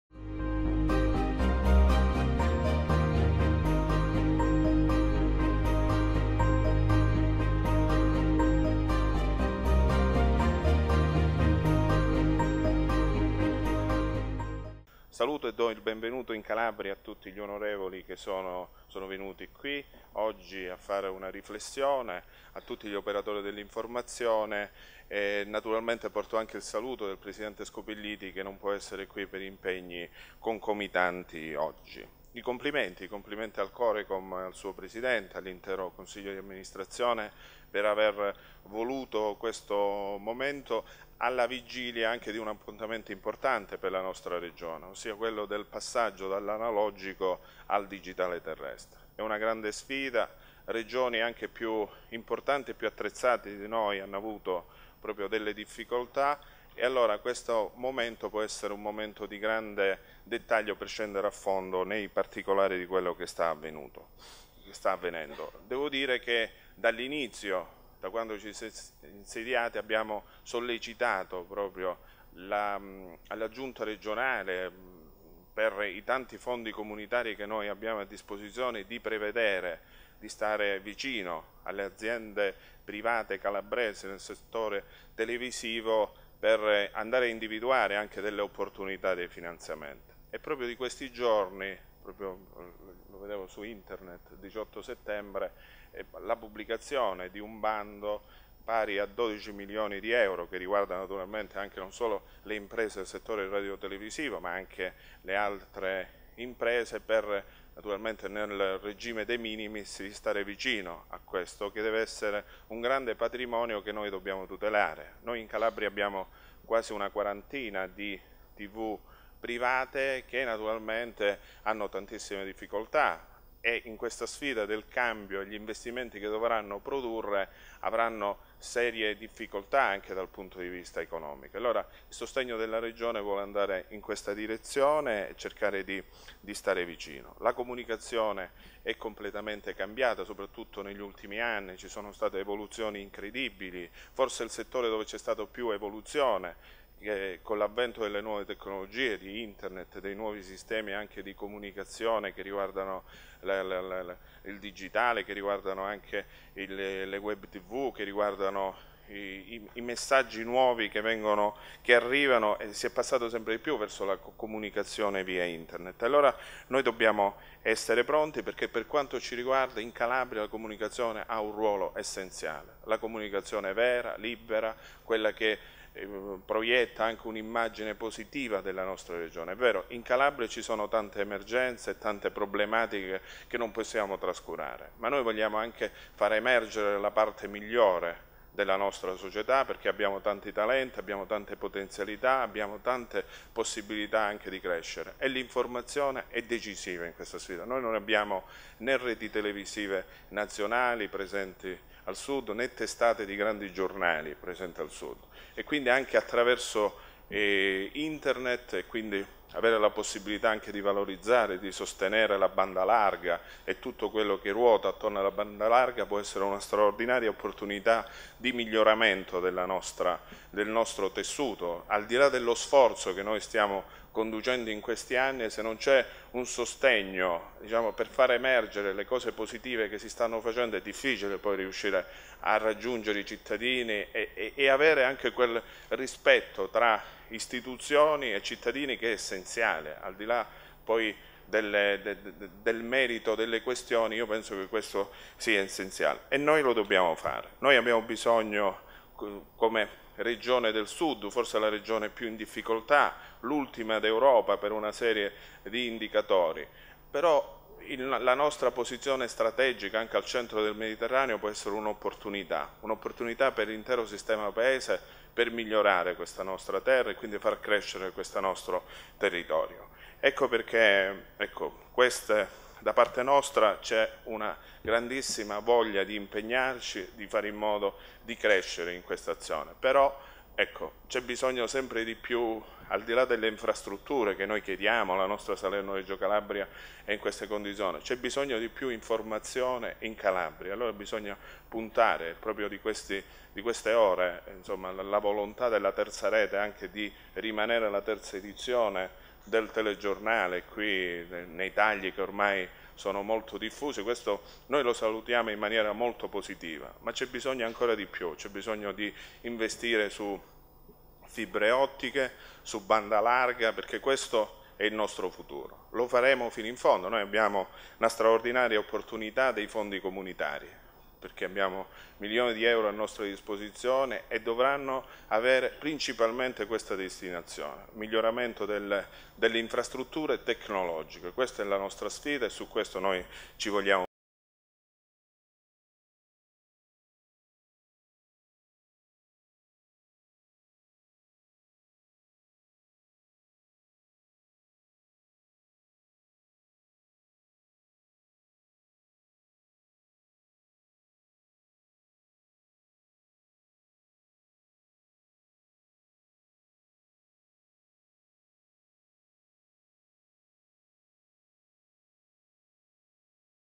Comunicazione e prospettive per il Sud, intervento di Francesco Talarico (Regione Calabria)
Francesco Talarico, Presidente Consiglio Regionale della Calabria, interviene al convegno "Le nuove frontiere della comunicazione: traguardi e prospettive per il Sud", promosso da Comitato regionale per le comunicazioni della Calabria a Palazzo Campanella, sede del Consiglio regionale della Calabria. Tra i principali temi in agenda, il Corecom Calabria ha scelto lo switch-off e tutte le questioni connesse allo spegnimento dell'analogico in una regione ortograficamente complessa come la Calabria.